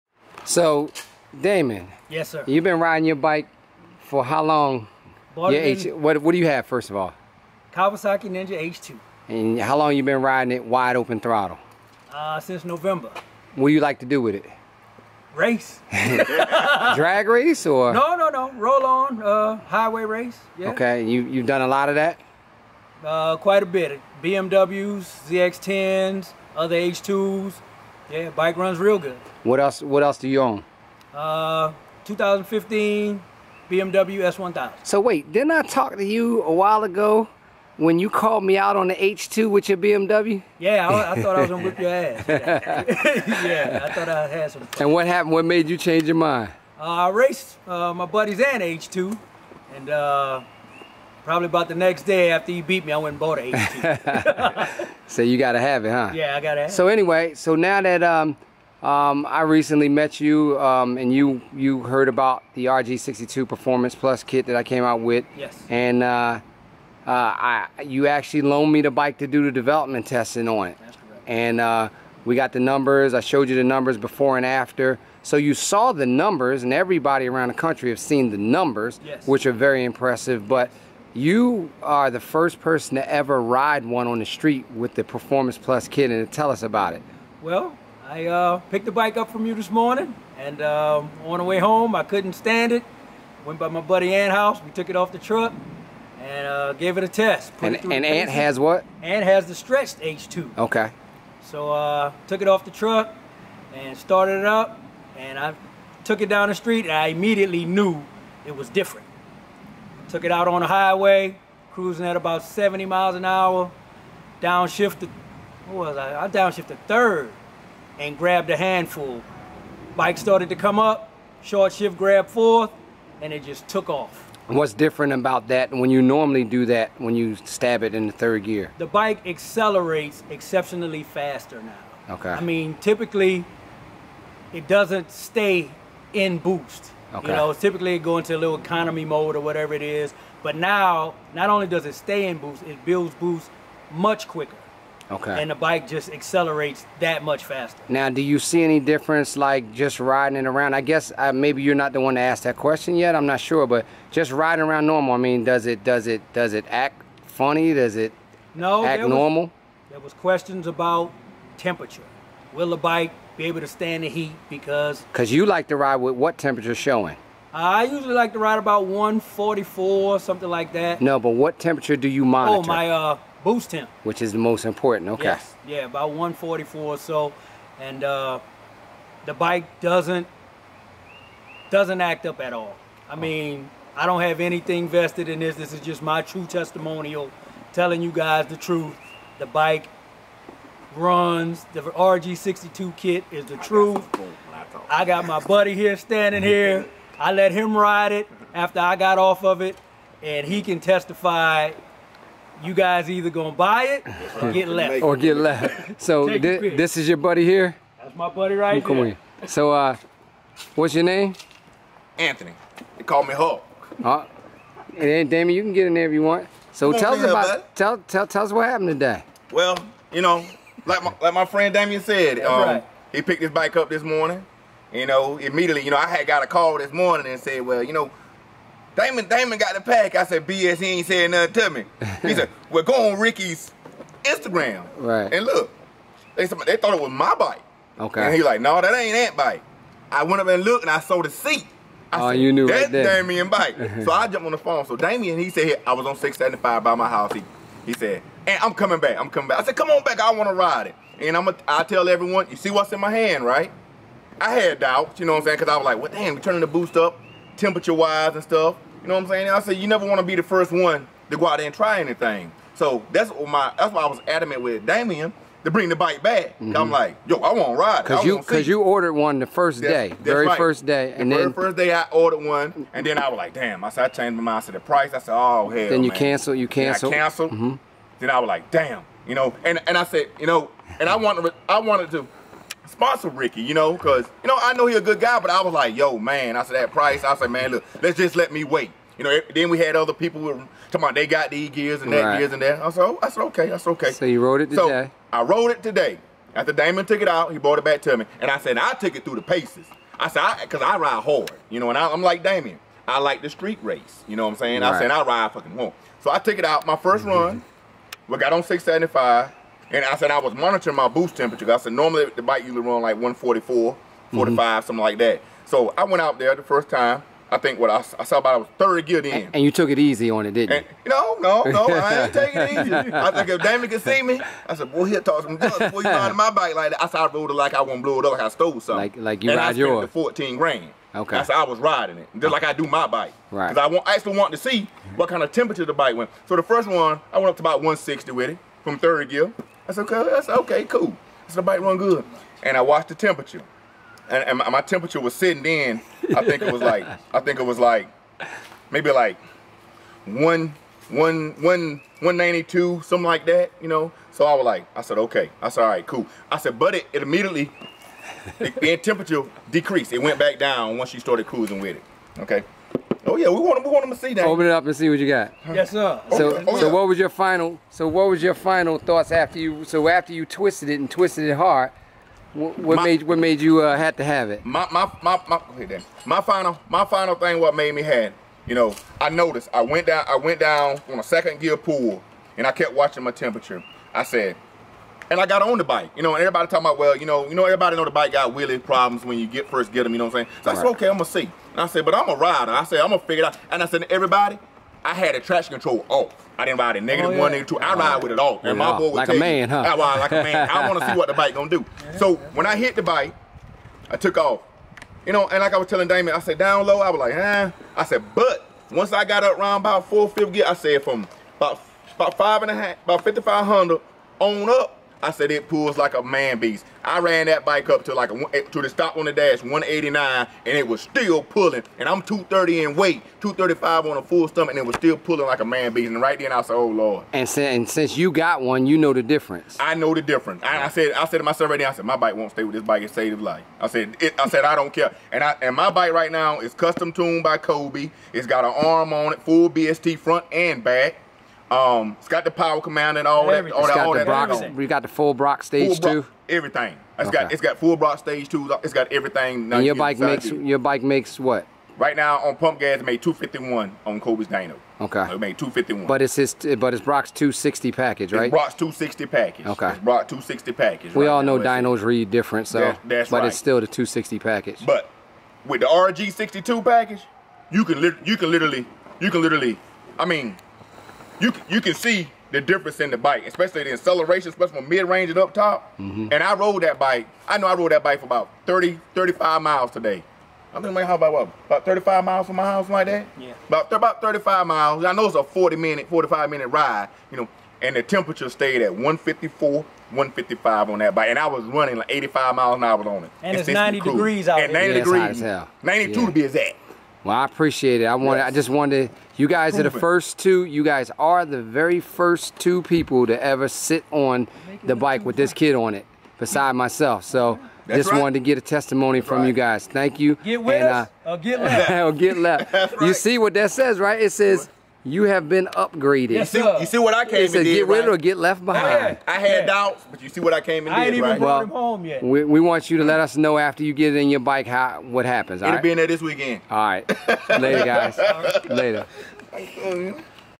2 happy H2 owners with Rickey Gadson RG62 Performance Plus kit installed on their Kawasaki H2
The first H2 owners in the US get to experience the absolute overwhelming power of the Rickey Gadson H2 upgrade kit installed on his H2 wanting to tell the world of their experience after the 1st ride.